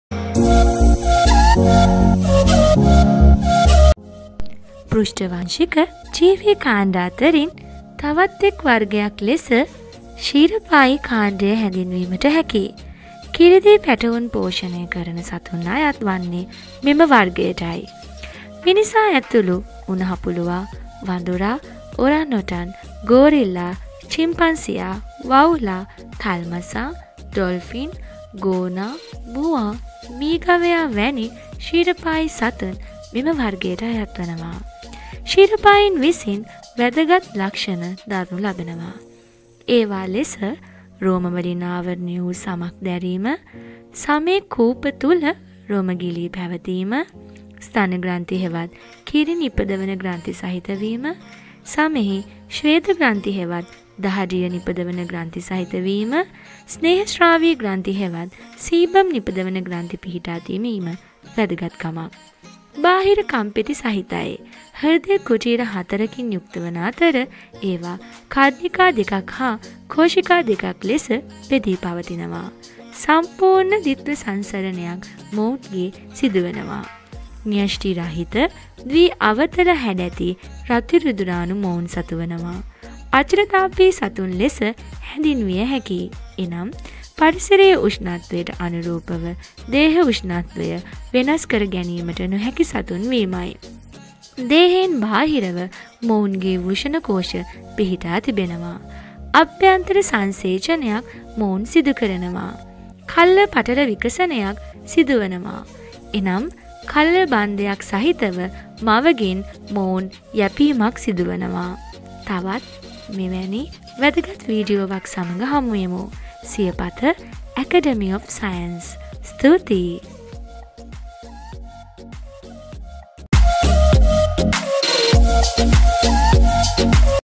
Mammalian | ක්ෂිරපායින් @Siyapatha Academy of Science 2020
ප්‍රධාන පෘෂ්ඨවංශික කාණ්ඩ අතුරින් ප්‍රධාන වර්ගය ක්ෂීරපායින් වේ.

ස්තූතියි..!